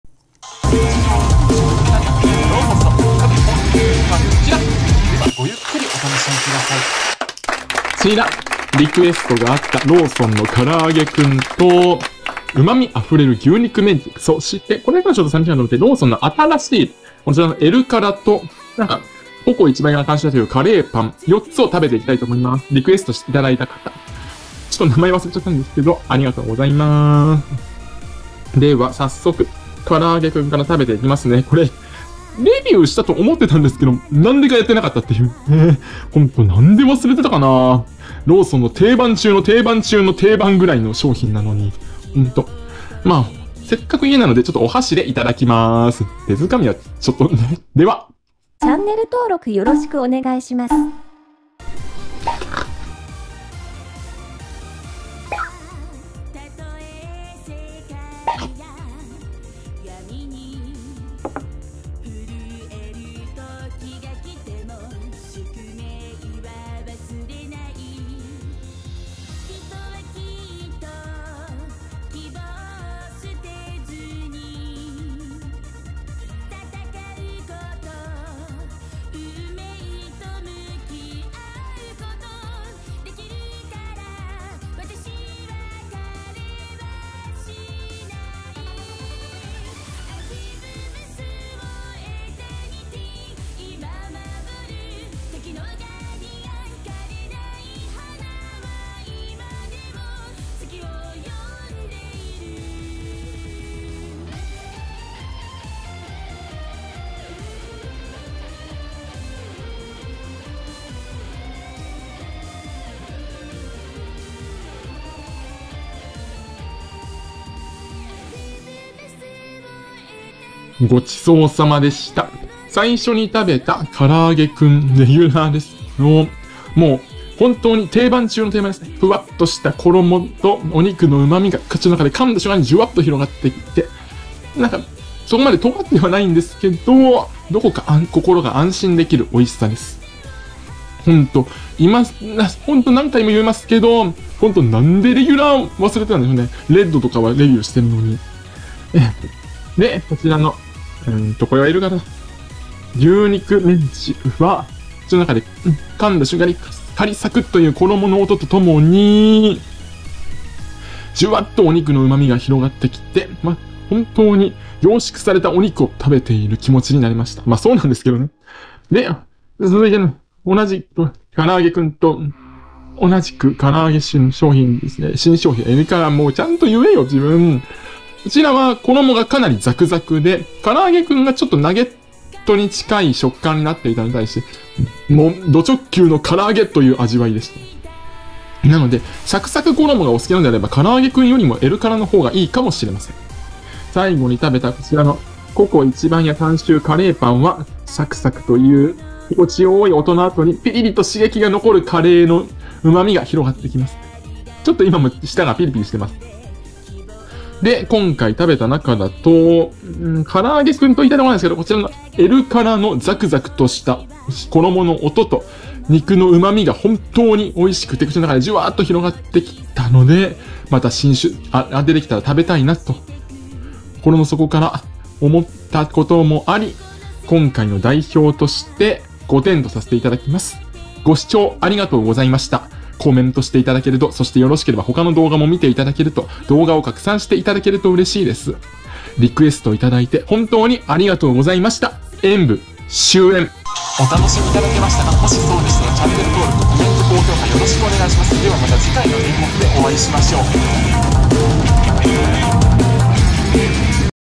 リクエストに応えてローソン商品食べてみたら～
「なぜこれを忘れていた？」
セブンに続いてローソンの◯◯も、
リクエストいただいたので食べてみました〜
しかしなぜ◯◯クンをレビューしてなかったのか、
一体どうしてだとあなたは思いますか〜

アマゾンほしい物リスト↓